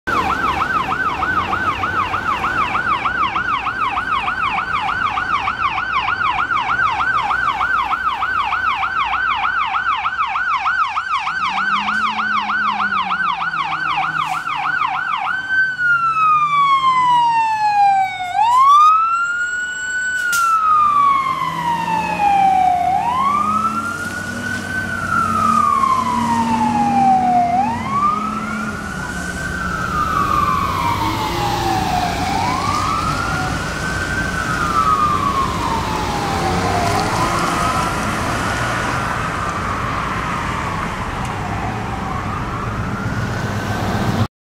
Minneapolis Fire - Engine 04 Responding - 09/25/17
MFD E-04 responding on a medical emergency. Enjoy!